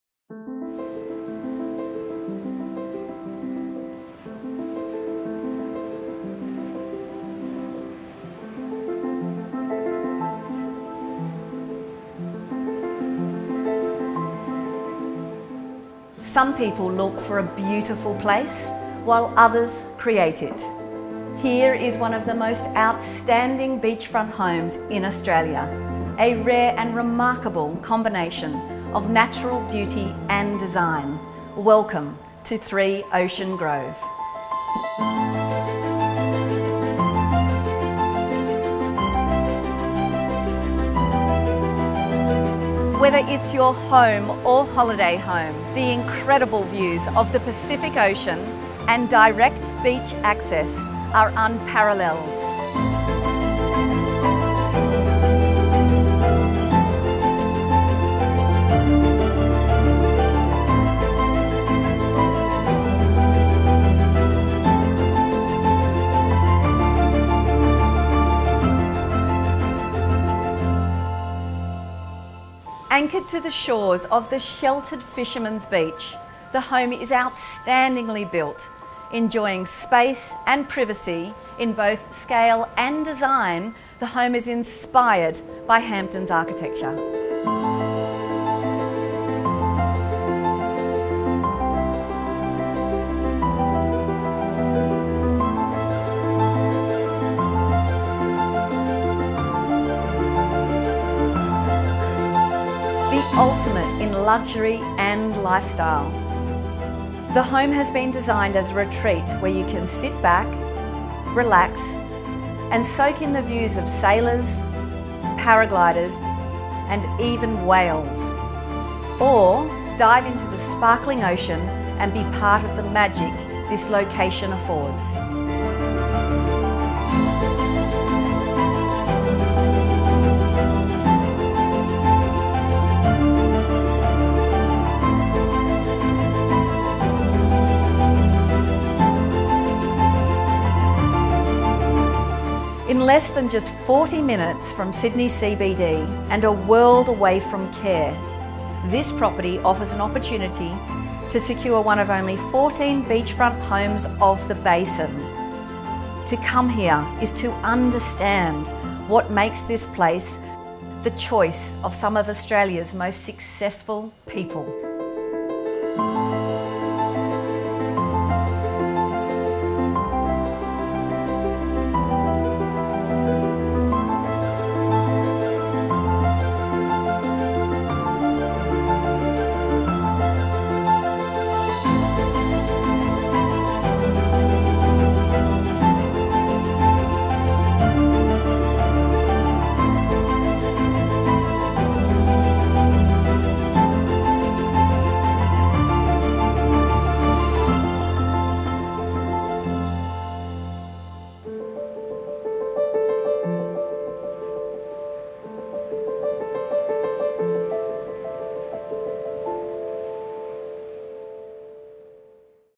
SOLD | 3 Ocean Grove, Collaroy | Clarke & Humel
Anchored to the shores of Fishermans Beach, one of Sydney's most sheltered beaches, with point blank ocean views providing a sublime backdrop, the architect-designed 'Mooncusser' has been brilliantly conceived to provide the ultimate trophy beach house. Drawing inspiration from Cape Cod and Hampton style design aesthetics with bespoke timber finishes and giant walls of glass, it soars over three exceptionally spacious floors that extend to seaside terraces and level lawns with direct beach access.

Michael Clarke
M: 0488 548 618

Garry Greco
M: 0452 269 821